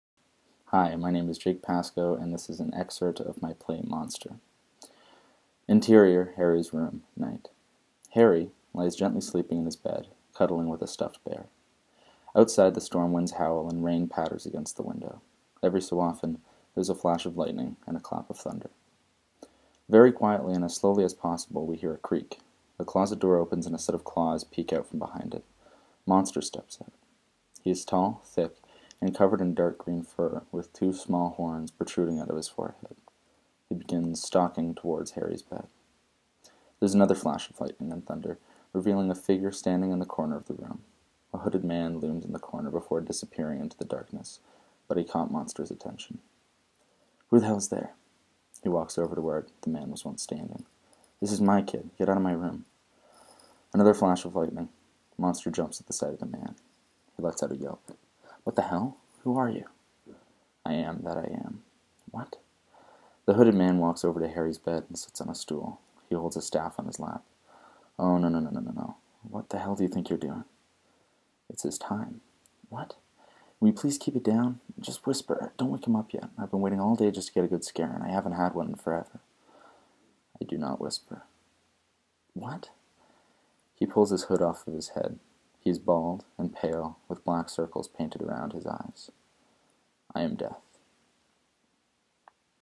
Follow the Playwright Mentorship Edition - Jake Pascoe: Monster
Jake Pascoe reads an excerpt from his script, Monster.